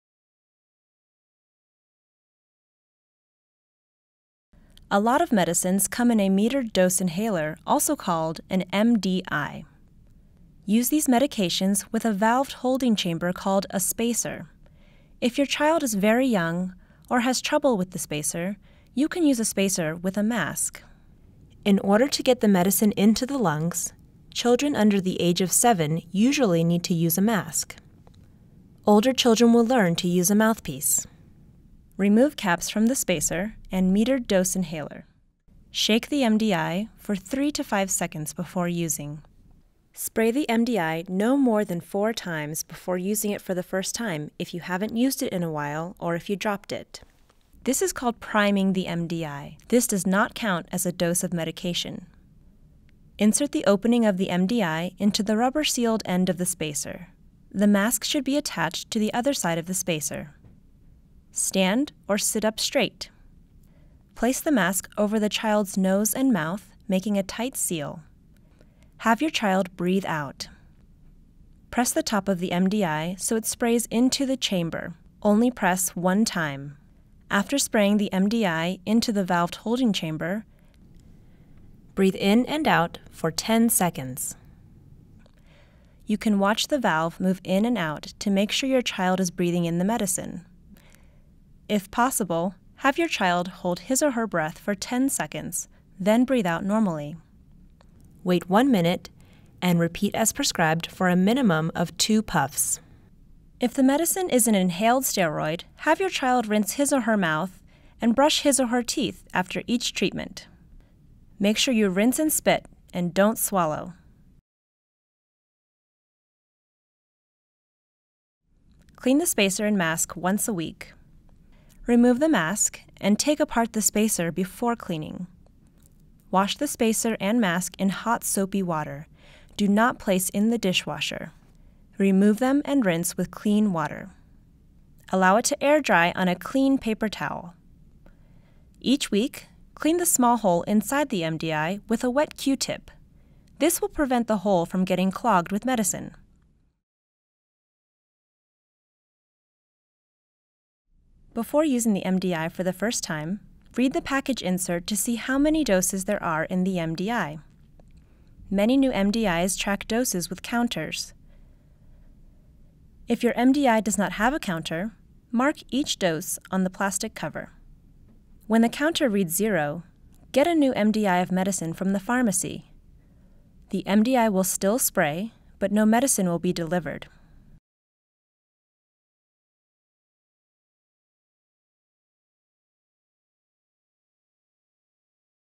How to Use an MDI With a Mask
An MDI Inhaler with a Spacer/ Holding chamber eases the delivery of medicine to the lungs. This makes the medication more effective. A facemask should be attached to the Spacer for children who cannot coordinate pressing down on the MDI, inhaling and holding their breath for a count of 10. Children should always use their Spacer/ Holding chamber with a metered dose inhaler (MDI).